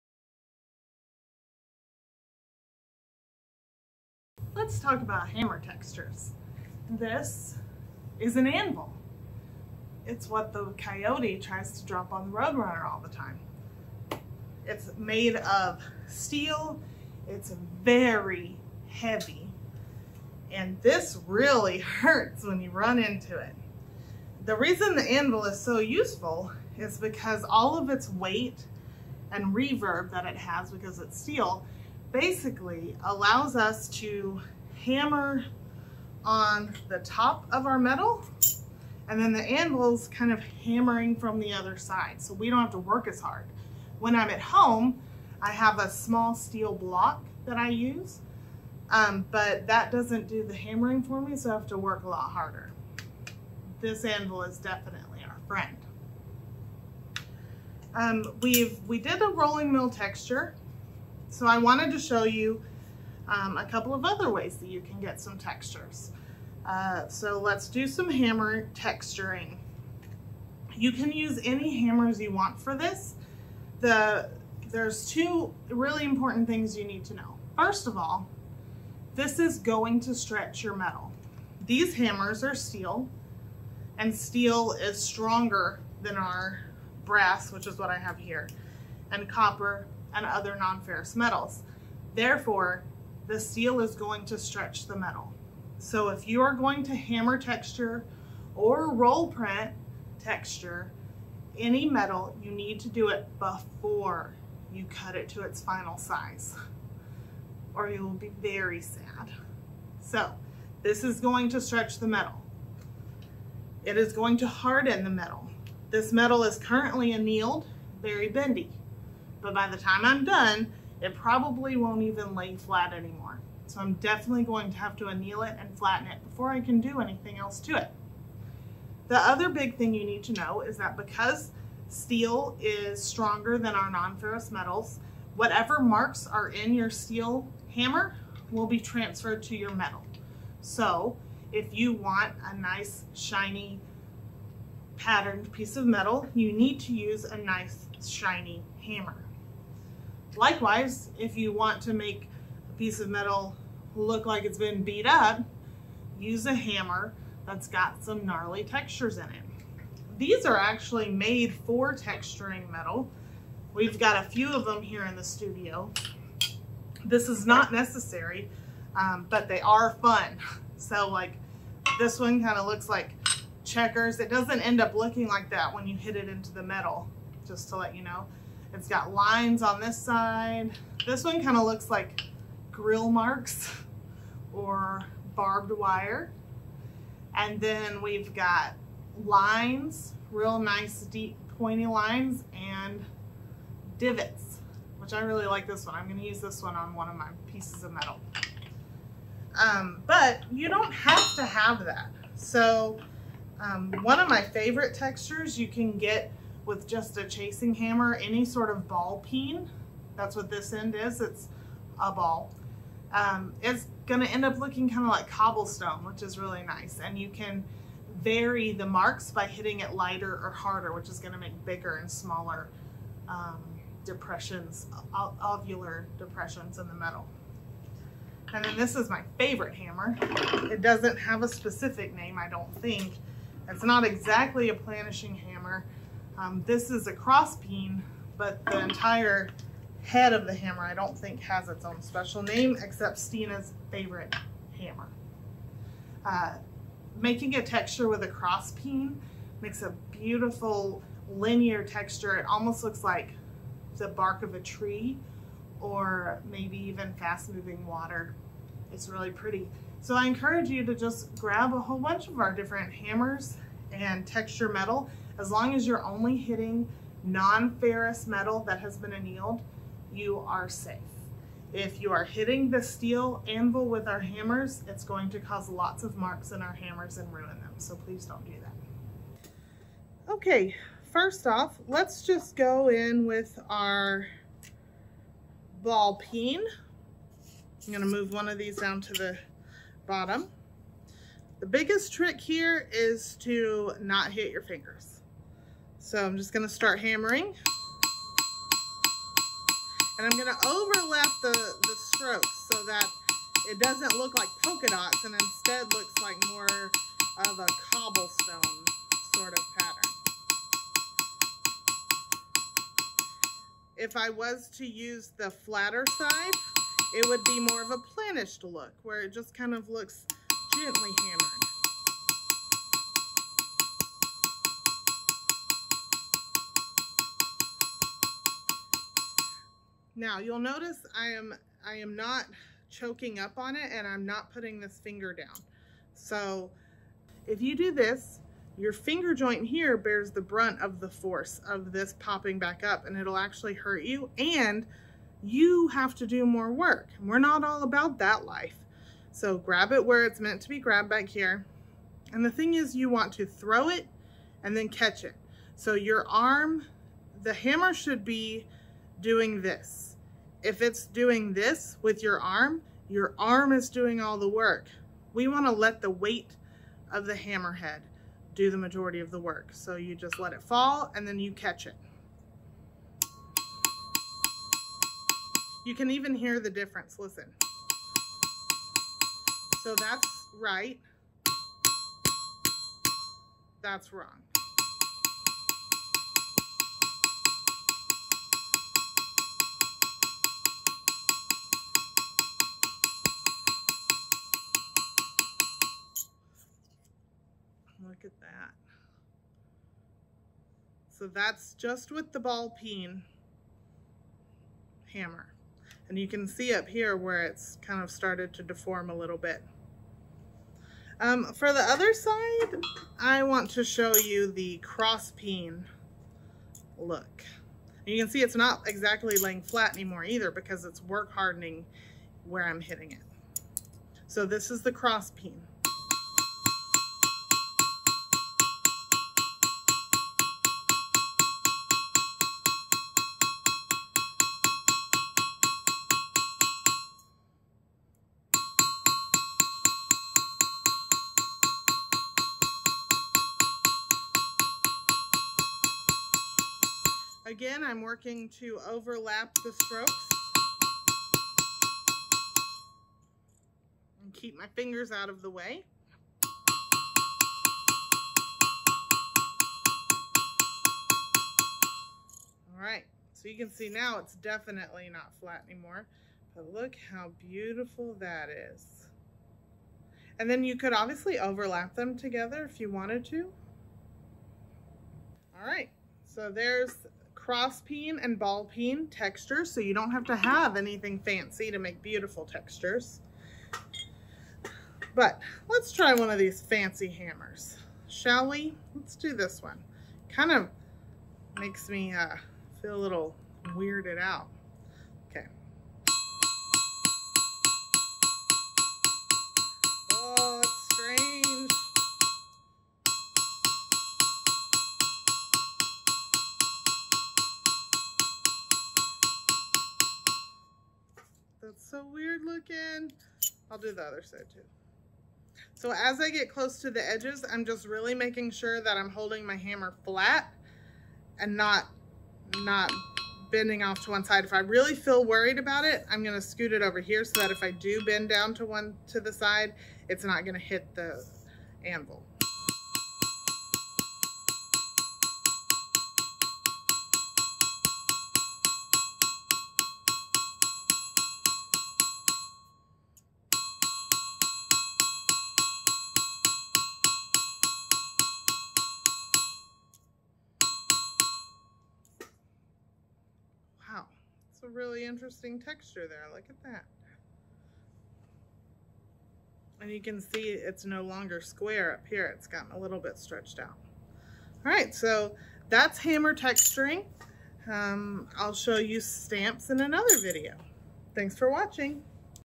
Hammer Texturing
Use hammers to make a multitude of textures in your metal